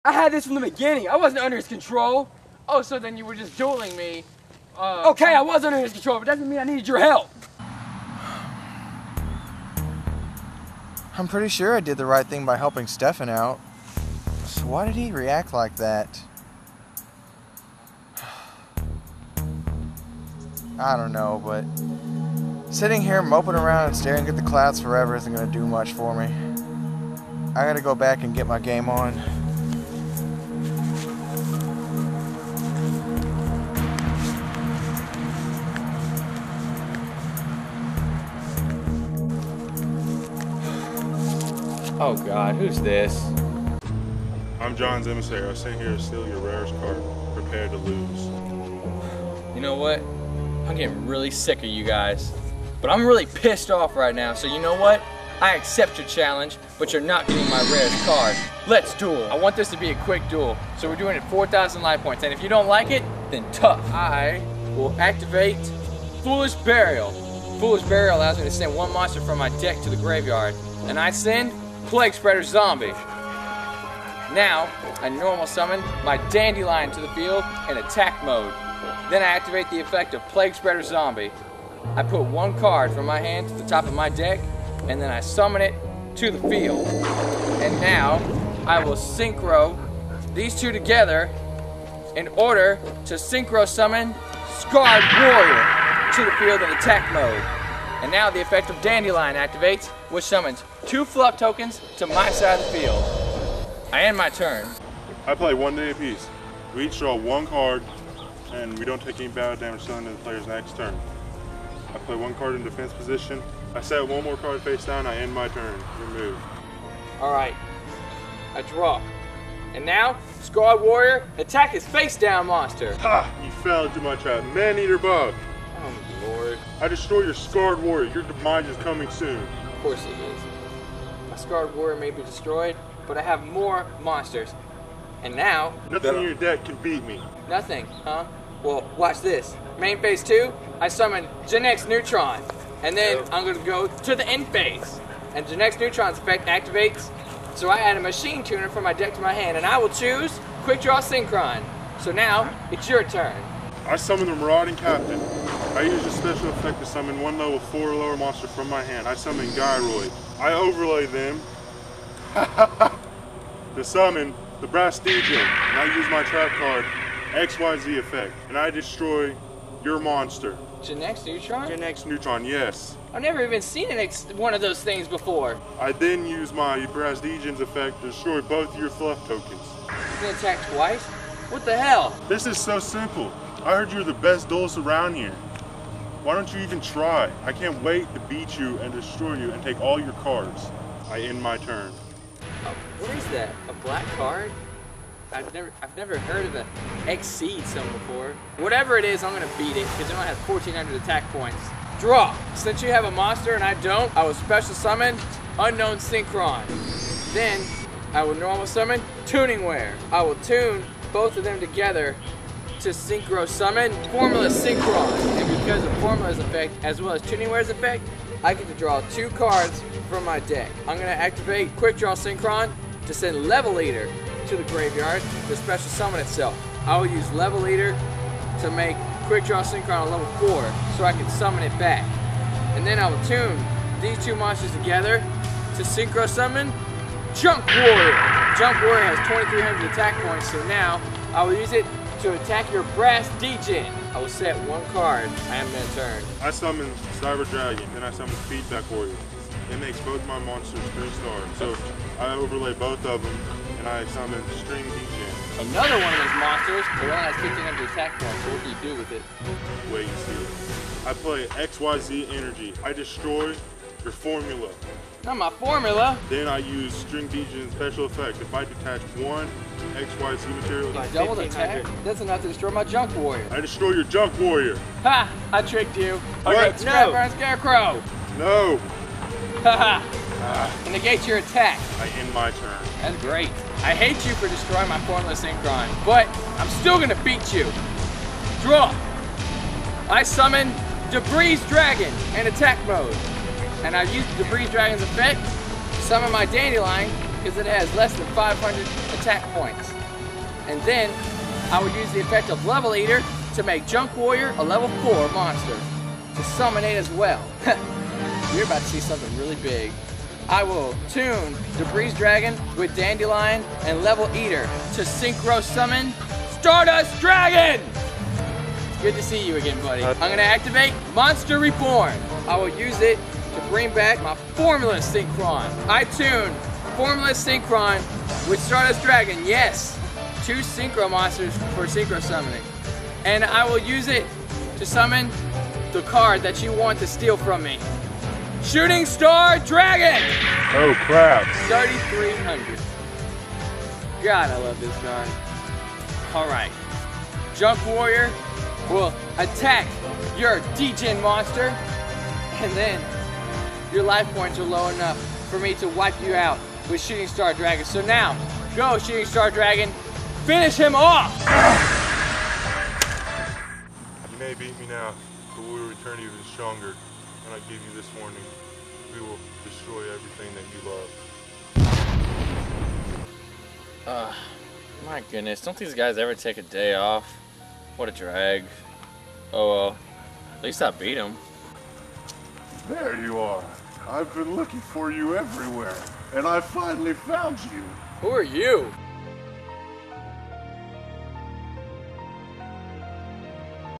YuGiOh Chronicles Episode 10 Part 1
I told you guys I would re-upload this, I just wasn't satisfied with it.

I figured out what was wrong with my editing software and why it was rendering my videos in bad quality. So now my episodes should start having better quality than they did before.

PLOT: after his last encounter with Stefan ends on a sour note, Zack is gazing at the clouds trying to reflect on what he did wrong. Eventually, he gets up and goes back to dueling. However, along the way, he is met by one of John's Shadow Duelist. who of course challenges him to a duel. Zack is still kind of irritated by his thoughts, and decides to accept the duel as a way of relieving his anger and crushing evil at the same time.